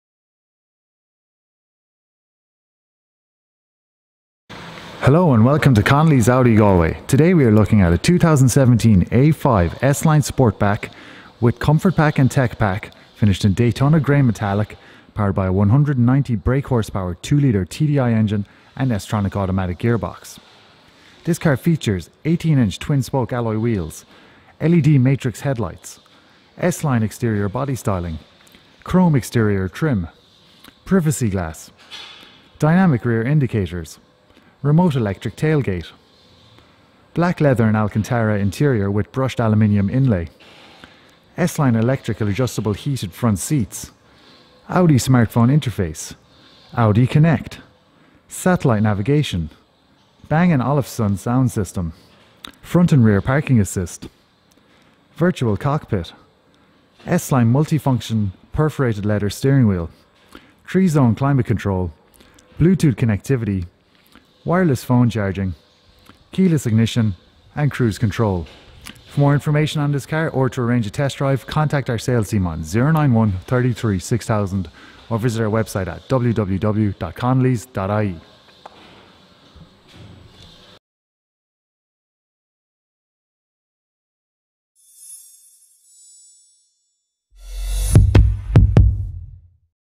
CMG AUDI GALWAY: 2017 A5 S LINE SPORTBACK 190BHP 2.0TDI AUTO *TECH PACK*
Stunning A5 S LINE SPORTBACK packed full of extras such as Virtual Cockpit, electric S Line sport seats, Sat Nav, LED Matrix Headlights and much much more! for more information contact our sales team on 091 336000 - FREE NATIONWIDE DELIVERY